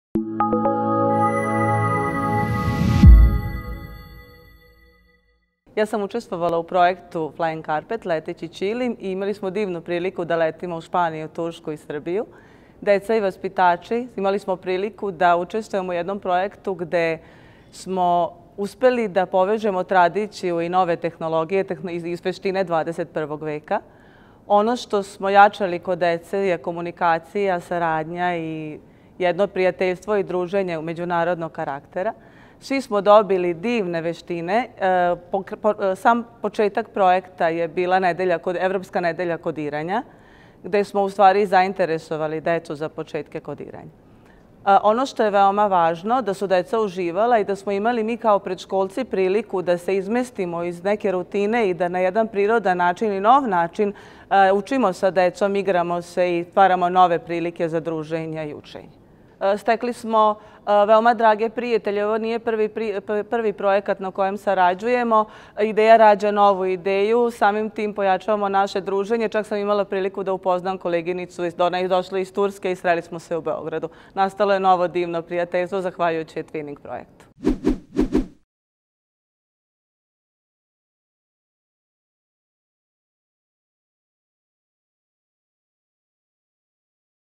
Evropske oznake kvaliteta: Flying Carpet
eTwinning projekat "Flying carpet" dobio je  evropsku oznaku kvaliteta za 2019. godinu.